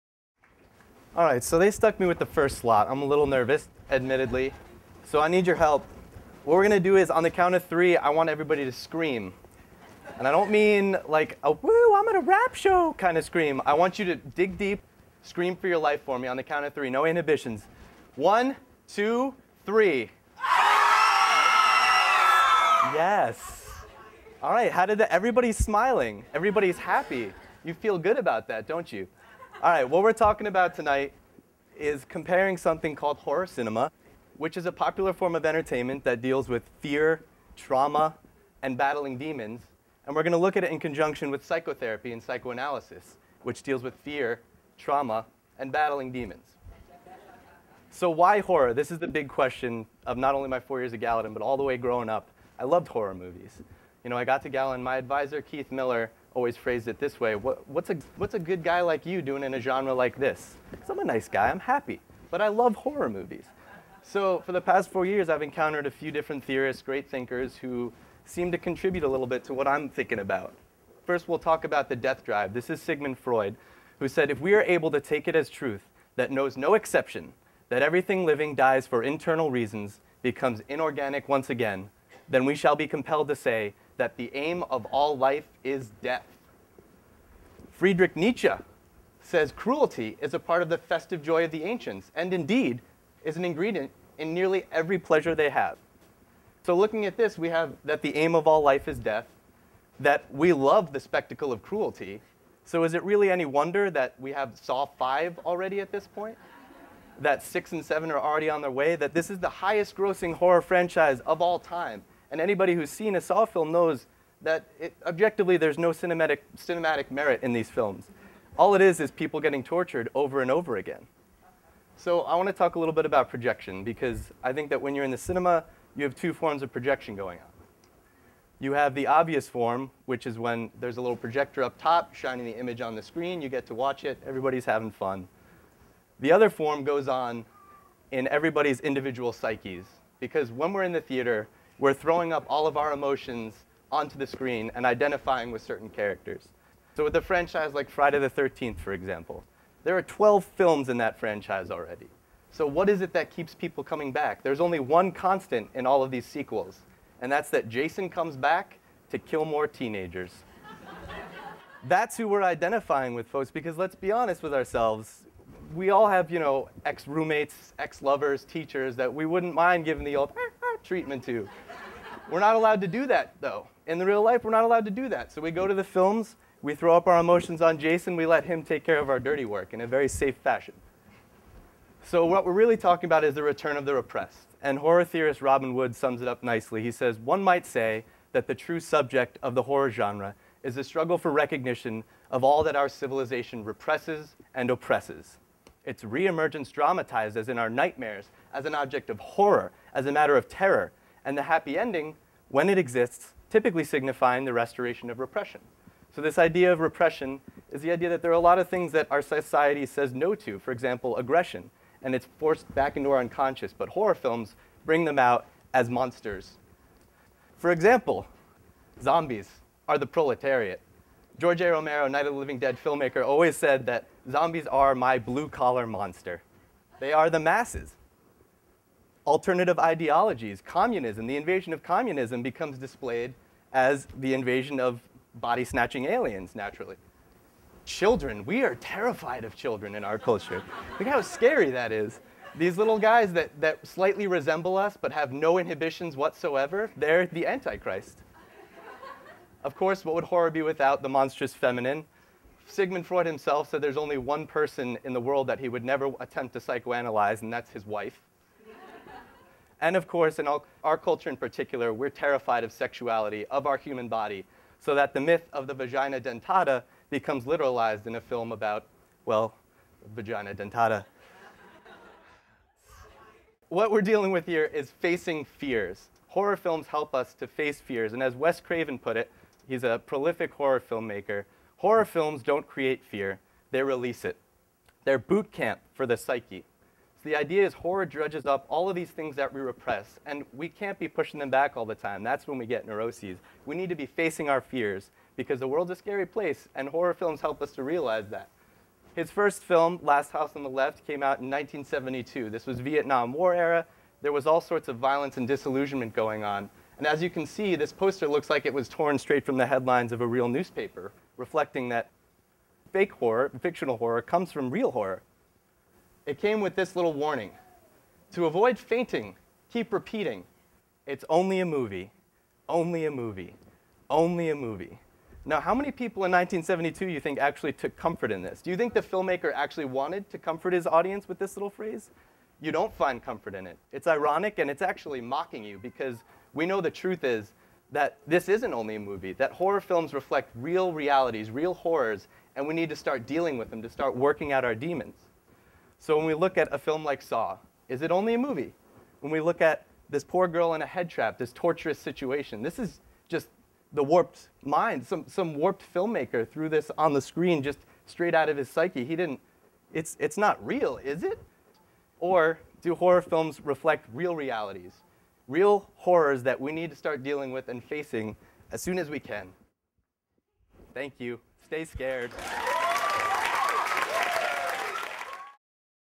Gallatin Senior Symposium: Brian McAuley: Horror Cinema and Psycho Therapy
The inaugural Gallatin Senior Symposium was held on April 20th, 2009 at New York Universitys Jerry H. Labowitz Theatre for the Performing Arts. Inspired by the annual TEDtalks, this TEDx event showcased twelve seniors from NYUs Gallatin School of Individualized Study, who were able to share inspiring stories and insights that developed from their unique concentrations in front of the public for the first time. The presentations topics span a range of artistic, cultural, and scientific genres, and highlight diversity of these academic scholars. Videos by Circa Production.